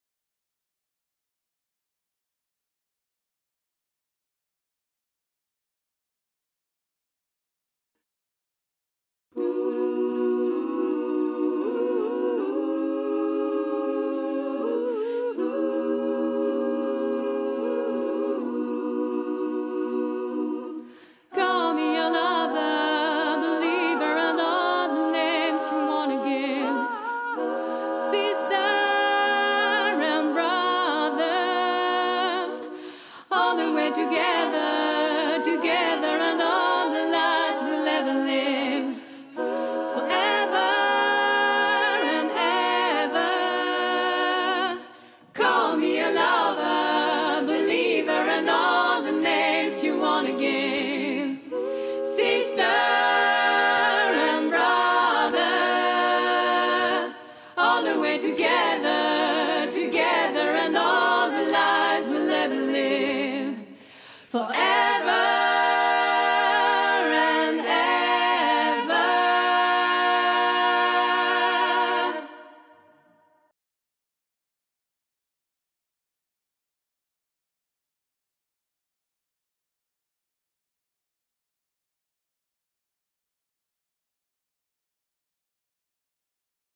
Bande-annonce Tsunami Bikini - La Mure Cinéma Théâtre
À l'occasion des Rencontres de la chanson, nous recevrons le vendredi 15 mai 2020 les Tsunami Bikini, pour notre plus grand bonheur ainsi que pour celui de nos oreilles !

4 voix, 4 personnalités artistiques vous invitent à surfer sur leur vague et découvrir des chansons de tous horizons sous un jour nouveau.

De Björk à Edith Piaf en passant par Radiohead, Brigitte Fontaine ou les Spice Girls, elles interprètent des morceaux connus ou méconnus. Autour de leur voix, les arrangements se teintent d’électro, de claviers planants et de rythmiques léchées pour révéler chaque titre.

Entre puissance et fragilité, profondeur et finesse, les Tsunami Bikini vous invitent le temps d’une traversée unique et sensible. Un raz de marée émotionnel et féminin qui risque fort de vous laisser quelques traces….